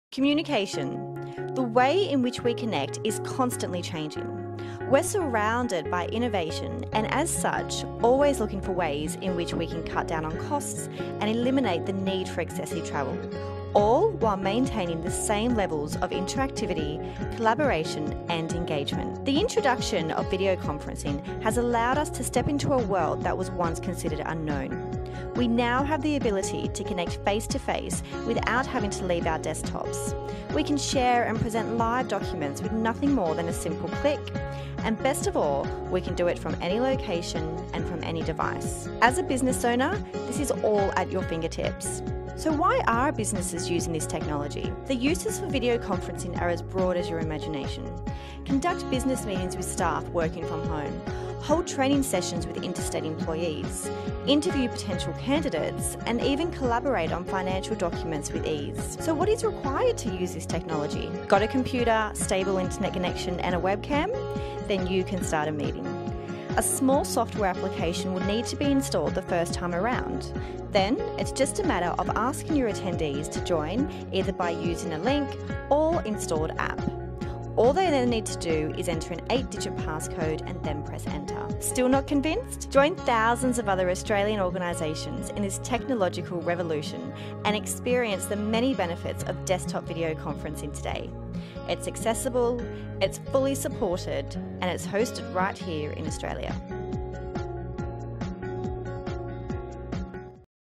Redback Videoconferencing - it's all at your fingertips!
| Cutting down on costs, eliminating unnecessary travel and all with a simple click? The future of desktop videoconferencing is here - it's accessible, it's fully supported and it's completely Australian!